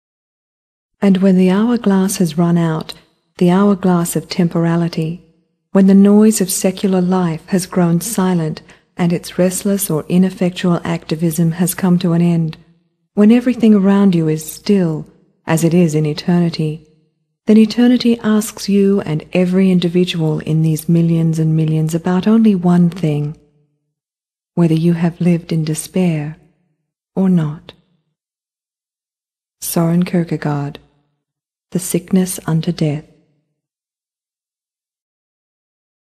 The Hourglass of Temporality
An excerpt from Soren Kierkegaard's 'The Sickness Unto Death'.